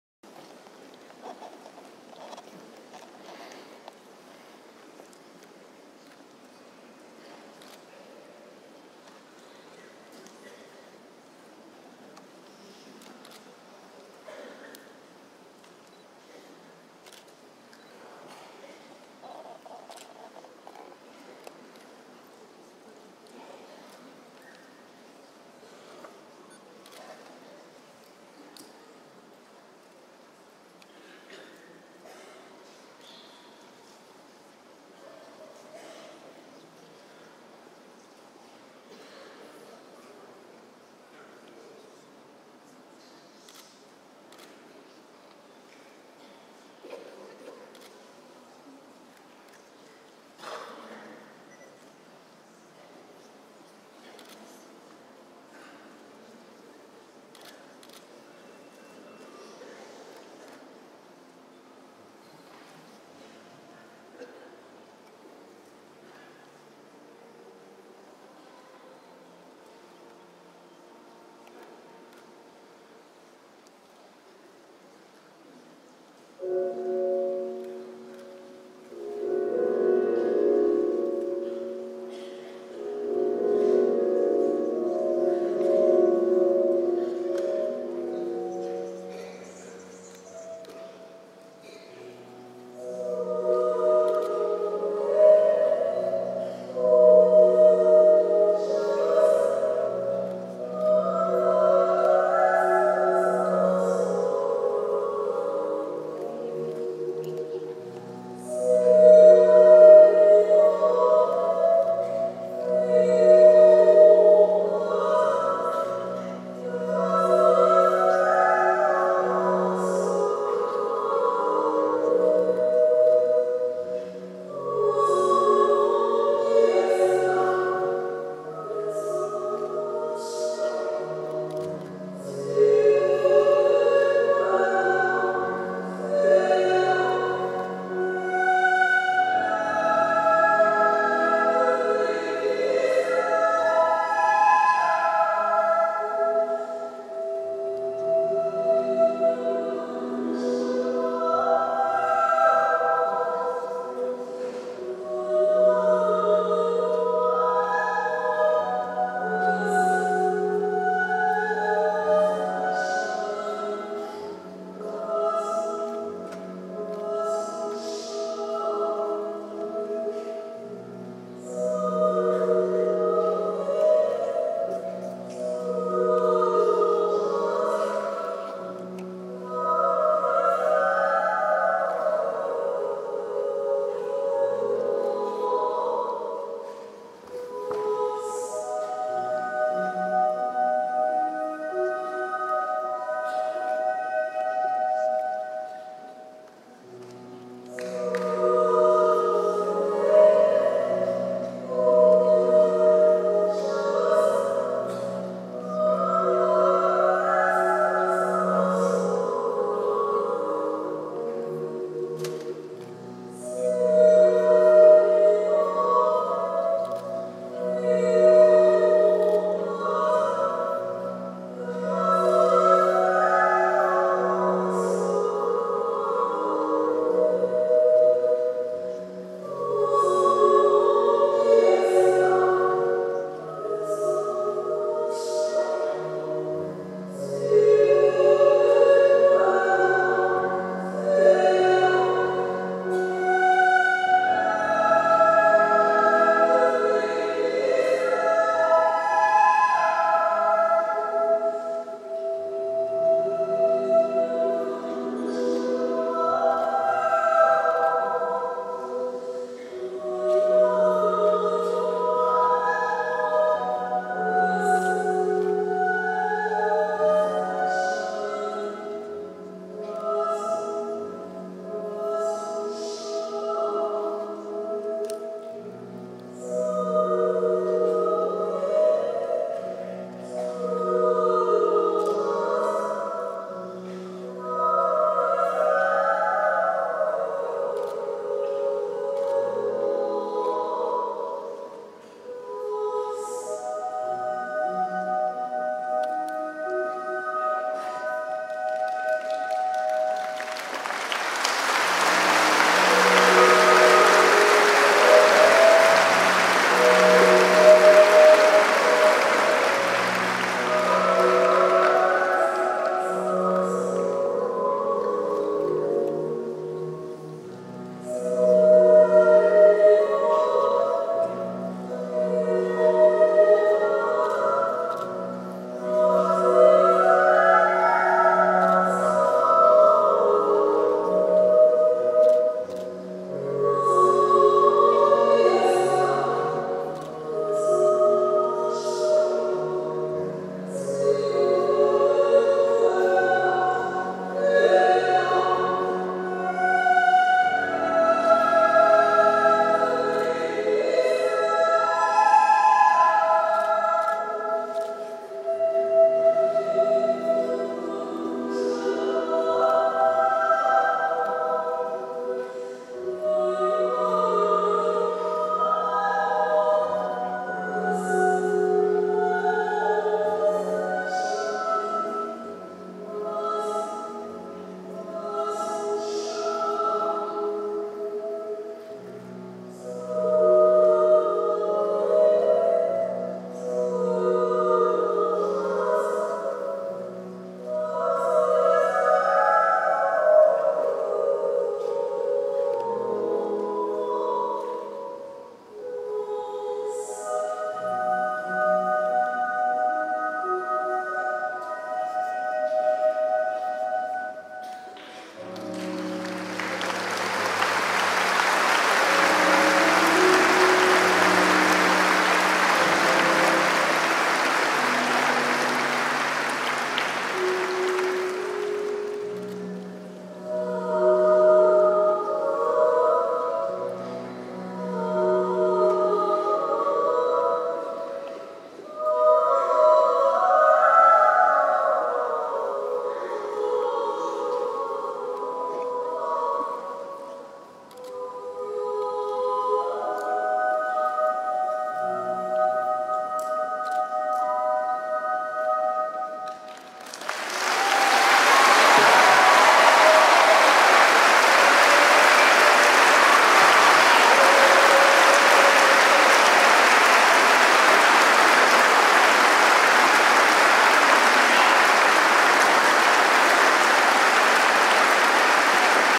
Diaconia Danse
Filmé à la soirée du 12 mai à Lourdes, danse voltige,